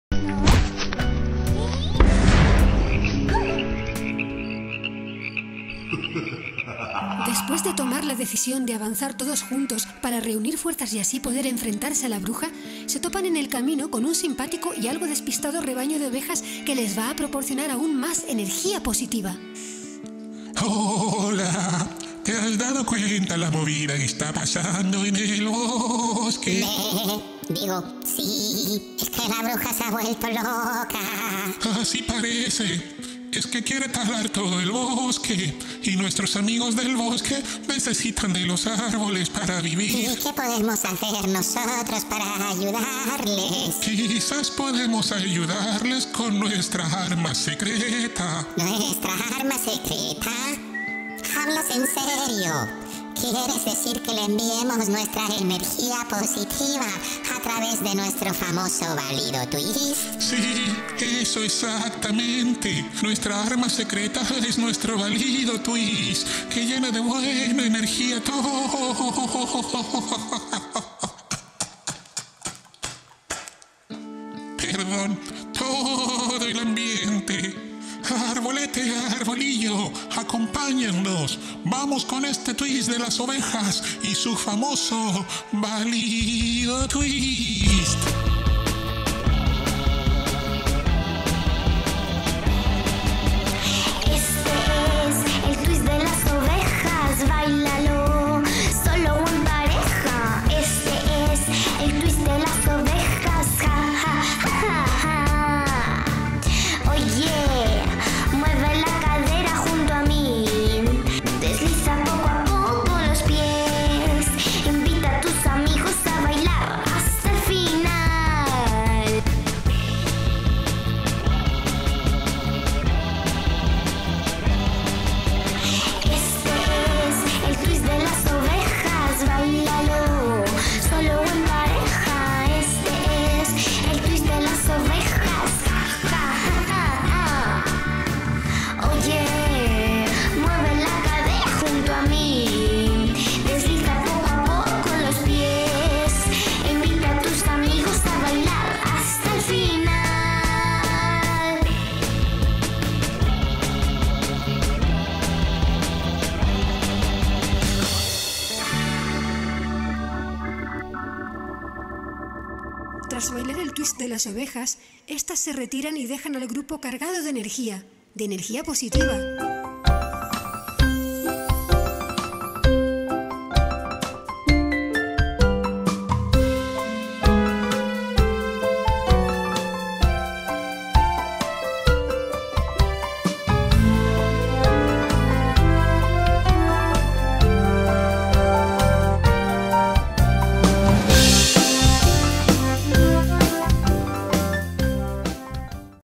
Capítulo 15 "UNAS OVEJAS CON MUCHA ENERGÍA POSITIVA"
Hoy más que nunca cobra valor la sinergia, la energía positiva.
En la avanzadilla hacia la Zona oscura habitada por la Bruja, los habitantes del bosque topan con un rebaño de ovejas algo despistado.
Estos animalitos están cargados de energía positiva que quieren transmitirnos a todos para que sumemos fuerzas y así enfrentarnos a la Bruja llegado el momento.
Canta con ellas la canción!!, haz bailar a tus padres este twist!!.
Agradecimientos a Eva Ivs por cantar esta canción y a Gloria, su mamá por hacer los coros. Mejor colaboración imposible!!.